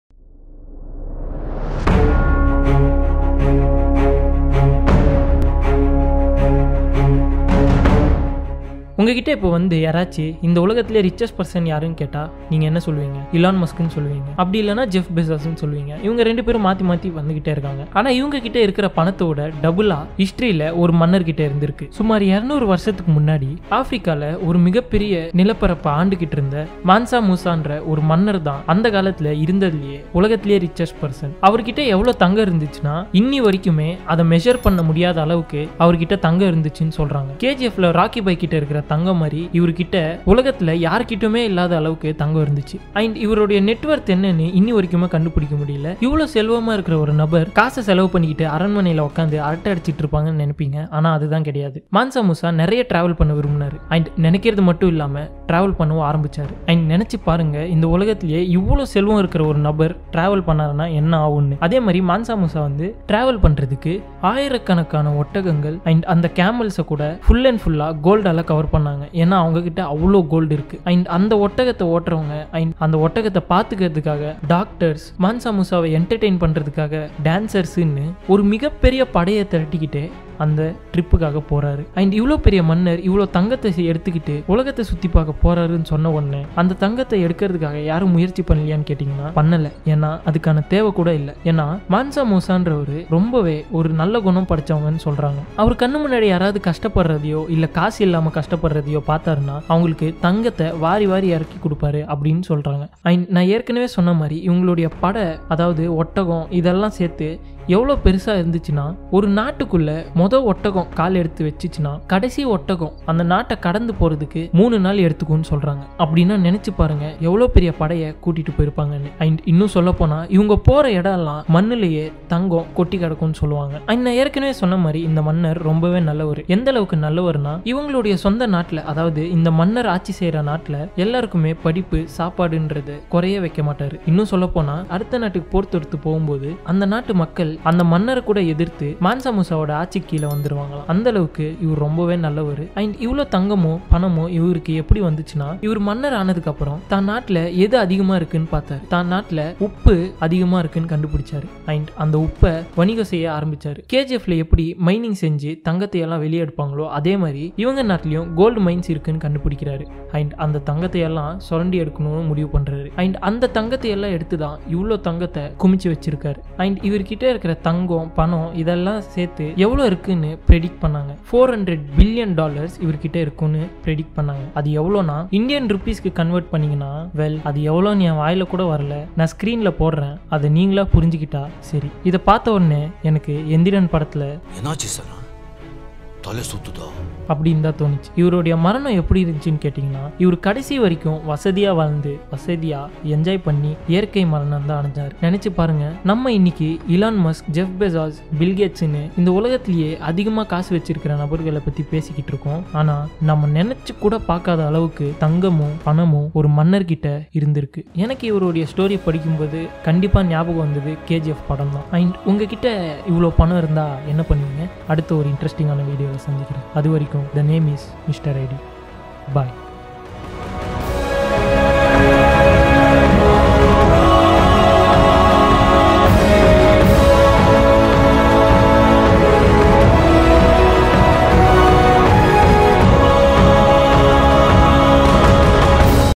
The Man Who Built El Dorado😱🤑 | Mansa Musa | Explained in Tamil | Mr ID
Hey Guys Here Some Facts About The Man Who Built El Dorado | Mansa Musa Explained In Tamil. Hope It Was Interesting And Informative😊.

Follow Our Channel (Mr ID) For More Informative ,Interesting And Funny Facts In Tamil

                                                            (Mr ID)